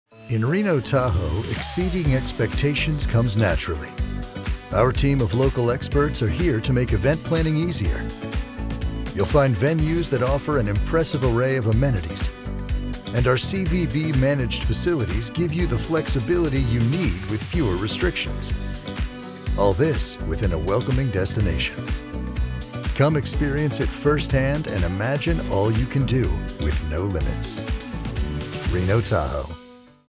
Reno Tahoe | Host Your Event in a Welcoming Destination
When it comes to creating one-of-a-kind events for attendees, Reno Tahoe is the place for you. Want to host a blacklight bowling party for 4,000 guests? Done. Dreaming of a sophisticated outdoor gaming soiree? No problem. How about a rooftop craft-beer tasting reception? Cheers. But this is just the beginning. Experience Reno Tahoe for yourself and start reimagining what’s possible with our Come See, Fly Free program.